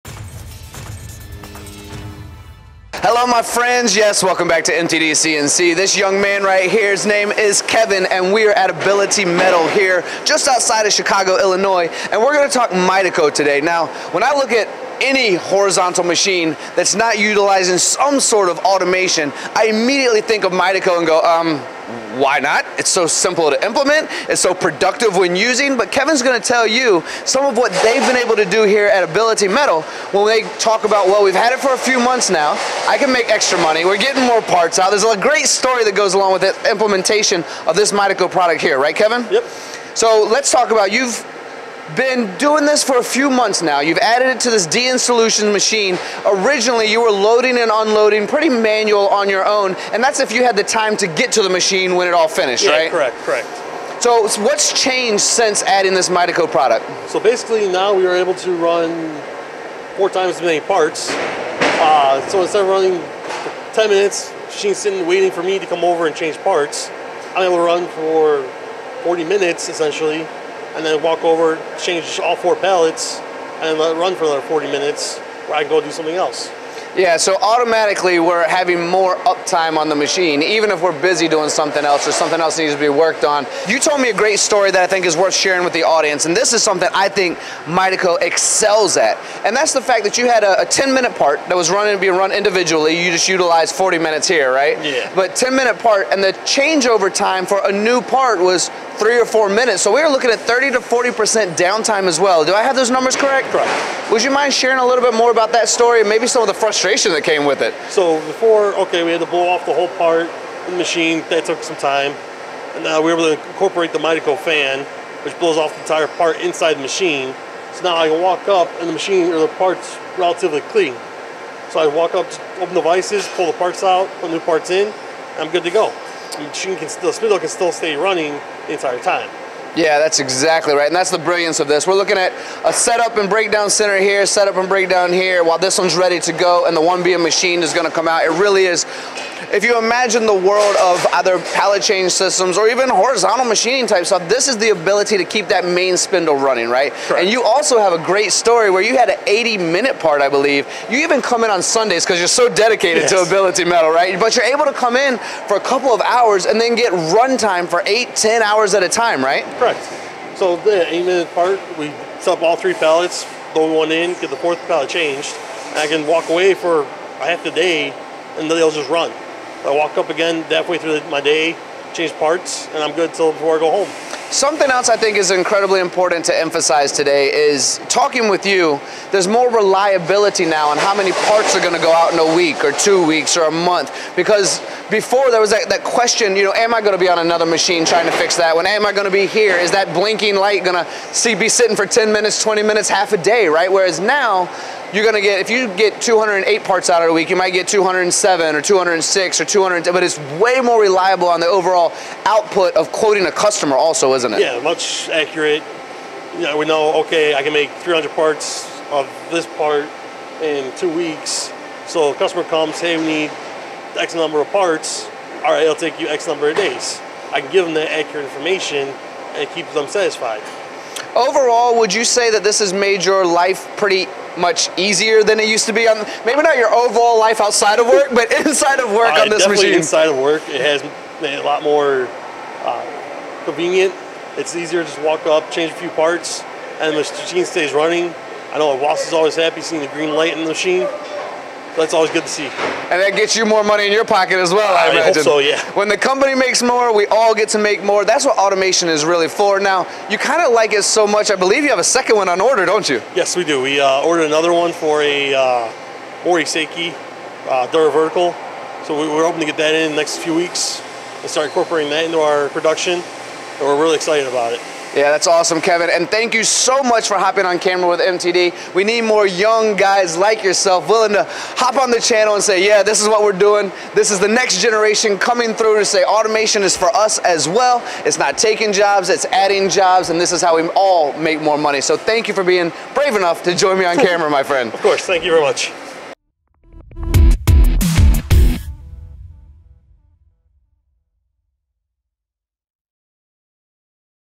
MIDACO Automatic 4-Pallet Pool on DN Solutions Doosan DNM 6700 - Ability Metal Testimonial
Ability Metal (formerly Ultra Specialties Inc), a nearly 100 year-old American manufacturing company based in Elk Grove Village outside of Chicago, needed to find an edge against competition that is growing more efficient by the day in making products for the aerospace and medical fields.

MIDACO’s Automatic 4-Pallet Pool made the difference with a significant increase in spindle utilization on their existing DN Solutions (Doosan) DNM-6700 from 2018.

Enjoy this interview with MTD CNC and learn how Ability Metal amplified their productivity with MIDACO.

For more info on MIDACO pallet changers

Video credits:
Tony Gunn & MTD CNC North America

Ability Metal (formerly Ultra Specialties Inc.)
1360 Howard St., Elk Grove Village, IL 60007